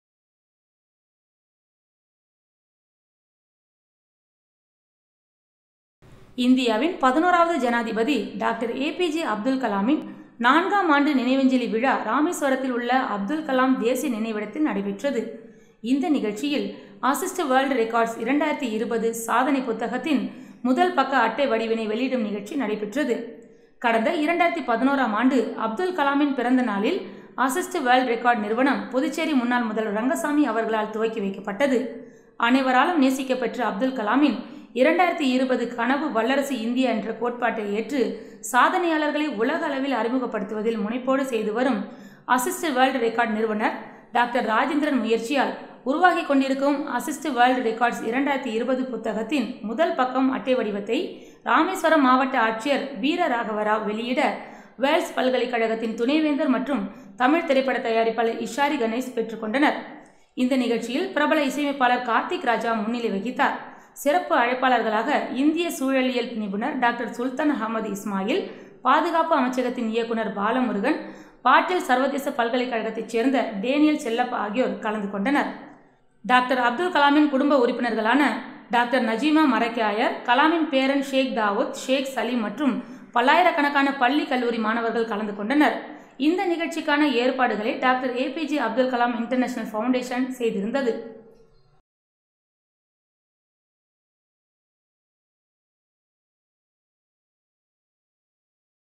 Assist World Records Book 2020 Cover Page Released IIFounder Dr.R.Rajendran
Tribute to the Dr.APJ Abdul Kalam 11th President of India...4th year Anniversary on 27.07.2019.organised by Dr.APJ Abdul Kalam International Foundation,Rameswaram,Tamilnadu,India.

Assist World Records 2020 Book Cover Page is Officially Launched on the 4th year Anniversary of our 11th President on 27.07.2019 at Dr.APJ Abdul Kalam National Memorial, Rameswaram. The first look poster was launched on a huge event amidst Students and Public by Honourable Collector of Rameswaram, Mr.K.Veera Ragava Rao IAS, Ishari.K.Ganesh Vice Chancellor of Vels University & Producer ,Kollywood Film Industry , Composer Karthik Raja, Dr.Sultan Ahmed Ismail (Indian Ecologist), Renowned ISRO Scientists, V.Balamurugan (Director-Ministry of Defence), Dr.J.Daniel Chellappa (VC-DY Patil International University, UC Berkeley), Kalam Sir’s Family, Dr.APJM Nazeema Maraikayar, APJMJ Sheik Dawood, APJMJ Sheik Saleem and other Delegates and showcased by Dr.APJ Abdul Kalam International Foundation (AKIF), Rameswaram .

ராமேஸ்வரத்தில் நடைபெற்ற மறைந்த முன்னாள் ஜனாதிபதி டாக்டர் ஏபிஜே அப்துல் கலாமின் நான்காம் ஆண்டு நினைவேந்தல் விழாவில்
"அசிஸ்ட் வேர்ல்டு ரெக்கார்ட்ஸ் 2020 "சாதனை புத்தகத்தின் முதல் பக்க அட்டை வடிவத்தை வெளியிடும் நிகழ்ச்சி நடைபெற்றது*

இந்தியாவின் 11-வது ஜனாதிபதி டாக்டர் ஏபிஜே அப்துல் கலாமின் நான்காம் ஆண்டு நினைவஞ்சலி விழா ராமேஸ்வரத்தில் உள்ள அப்துல் கலாம் தேசிய நினைவிடத்தில் நடைபெற்றது.
இந்த நிகழ்ச்சியில் "அசிஸ்ட் வேர்ல்டு ரெக்கார்ட்ஸ் 2020" சாதனை புத்தகத்தின் முதல் பக்க அட்டை வடிவத்தின் வெளியிடும் நிகழ்ச்சி நடைபெற்றது.
கடந்த  2011ஆம் ஆண்டு அப்துல் கலாமின் பிறந்த நாளில் அசிஸ்ட் வேர்ல்ட் ரெக்கார்ட் நிறுவனம் புதுச்சேரி முன்னாள் முதல்வர் ரங்கசாமி அவர்களால் துவக்கி வைக்கப்பட்டது.
அனைவராலும் நேசிக்கப்பெற்ற அப்துல் கலாமின் 2020 கனவு வல்லரசு இந்தியா என்ற கோட்பாட்டை ஏற்று சாதனையாளர்களை உலக அளவில் அறிமுகப்படுத்துவதில் முனைப்போடு செய்துவரும் அசிஸ்ட் வேர்ல்ட் ரெக்கார்ட் நிறுவனர் டாக்டர் ராஜேந்திரன் முயற்சியால் உருவாக்கிக் கொண்டிருக்கும் அசிஸ்ட் வேர்ல்டு ரெக்கார்ட்ஸ் 2020 புத்தகத்தின் முதல் பக்கம் அட்டை வடிவத்தை ராமேஸ்வரம் மாவட்ட ஆட்சியர் veera ragava raj வெளியிட வேல்ஸ் பல்கலைக்கழகத்தின் துணைவேந்தர் மற்றும் தமிழ் திரைப்பட தயாரிப்பாளர் இஷாரி கணேஷ் பெற்றுக்கொண்டார்.

இந்த நிகழ்ச்சியில் பிரபல இசையமைப்பாளர் கார்த்திக்ராஜா முன்னிலை வகித்தார்.
சிறப்பு அழைப்பாளர்களாக இந்திய சூழலியல் நிபுணர் டாக்டர் சுல்தான் அகமது இஸ்மாயில், பாதுகாப்பு அமைச்சகத்தின் இயக்குனர் பாலமுருகன், பாட்டில் சர்வதேச பல்கலைகழகத்தை சேர்ந்த டேனியல் செல்லப்பா ஆகியோர் கலந்து கொண்டனர்.

டாக்டர் அப்துல் கலாமின் குடும்ப உறுப்பினர்களான டாக்டர் நஜிமா மரைக்காயர் கலாமின் பேரன் ஷேக் தாவூத், ஷேக் சலிம் மற்றும் பல்லாயிரக்கணக்கான பள்ளி கல்லூரி மாணவர்கள் கலந்து கொண்டனர்.
இந்த நிகழ்ச்சிக்கான ஏற்பாடுகளை டாக்டர் ஏபிஜே அப்துல்கலாம் இன்டர்நேஷனல் பவுண்டேஷன் செய்திருந்தது